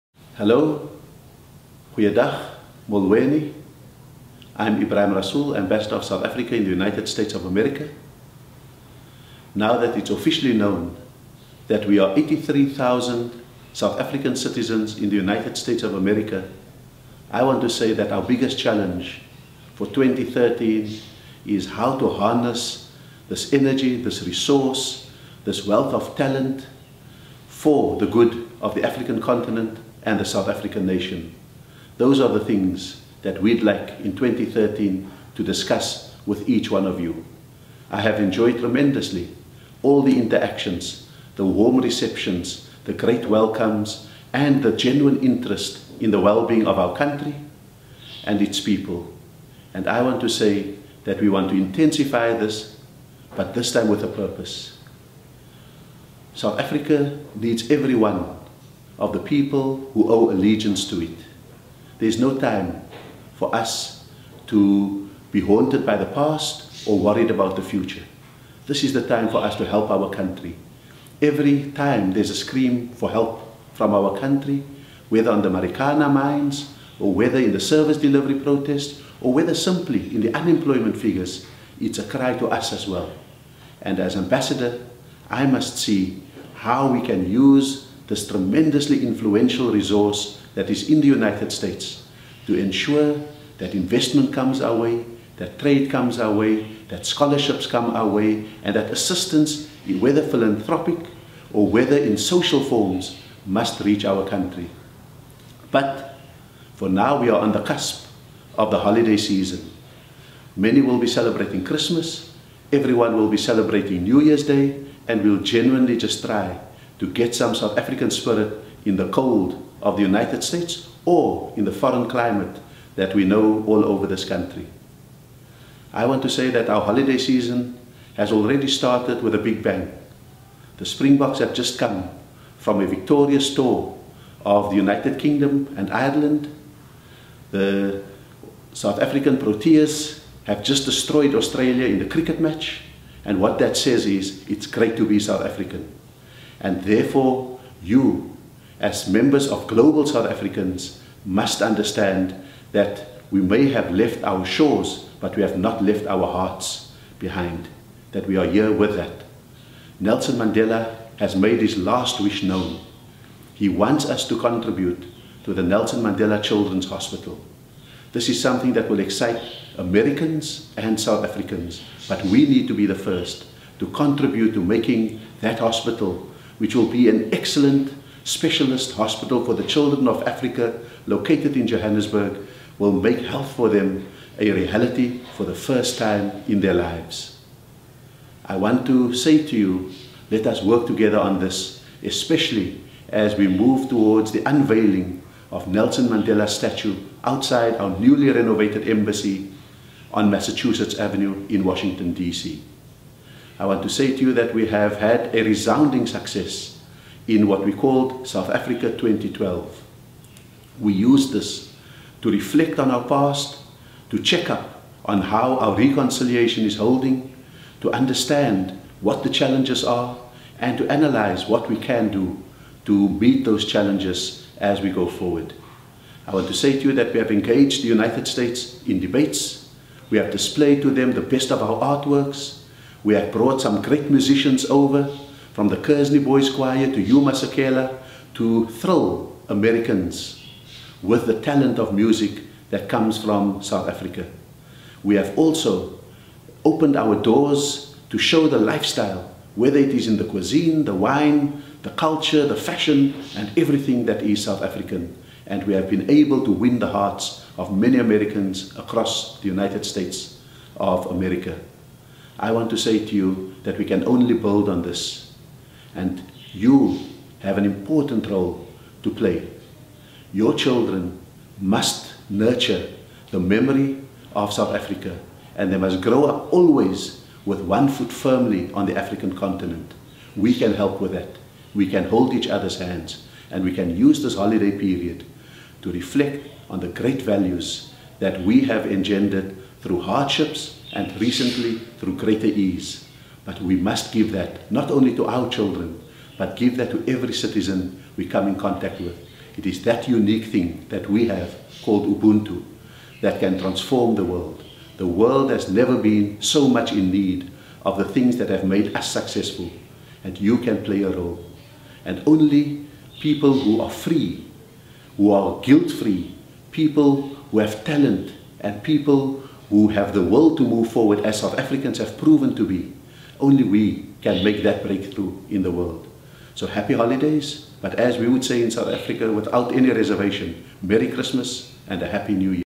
A Holiday Message to South Africans in the US from Ambassador Ebrahim Rasool
South Africa's ambassador to the US, Ebrahim Rasool, reaches out to the 83 000 South African the US census says are living in the US, wishing them a 2012 happy holiday season and prosperous 2013.